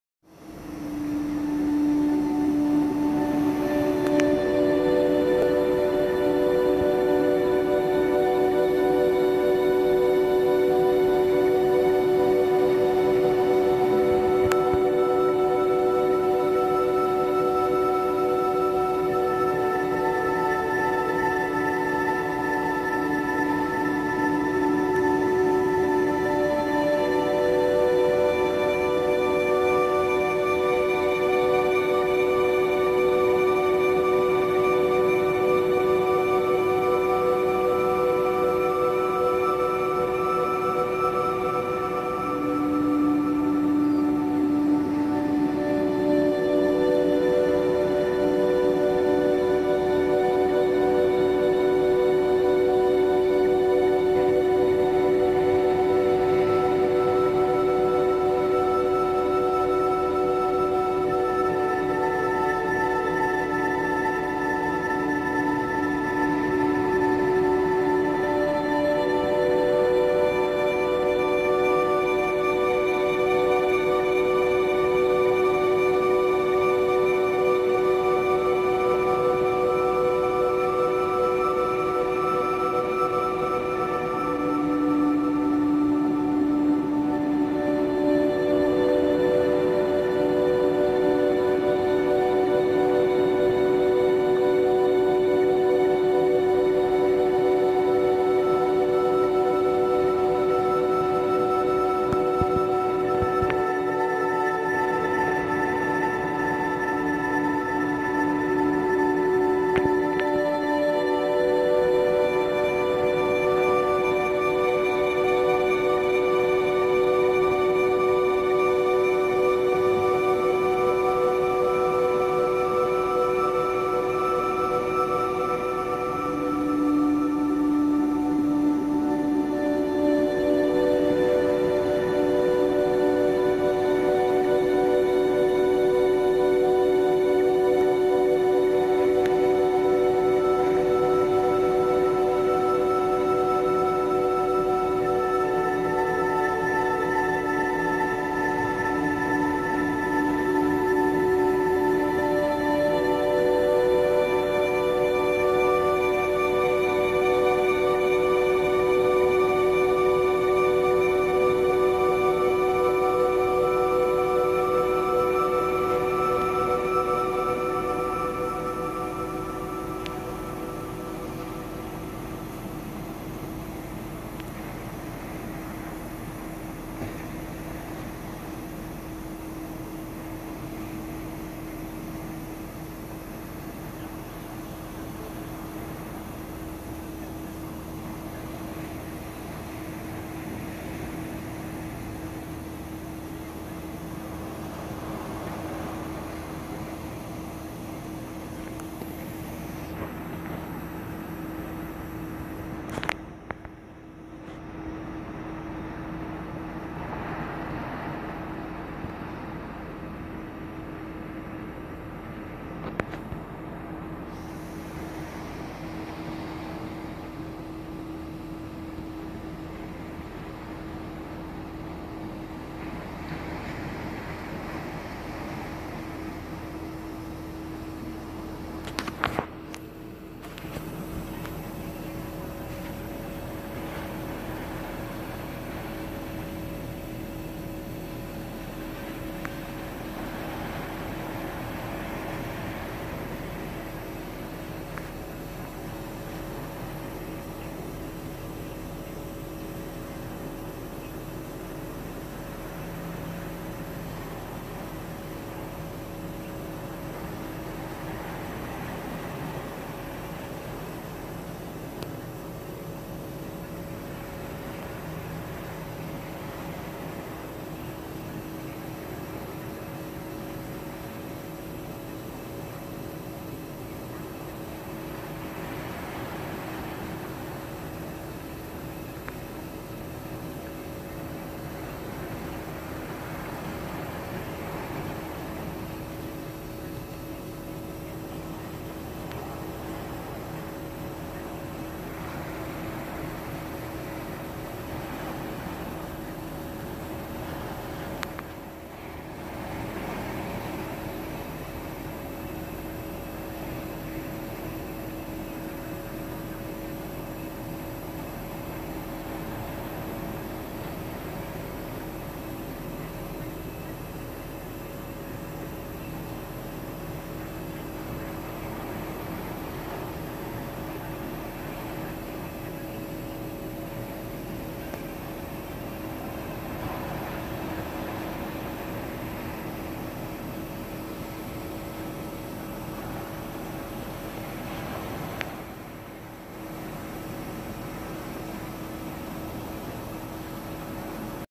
Daybreak at the Virginia Beach oceanfront - Dolphin Watching From Marjac Hotel & Suites
Dolphin Watching at daybreak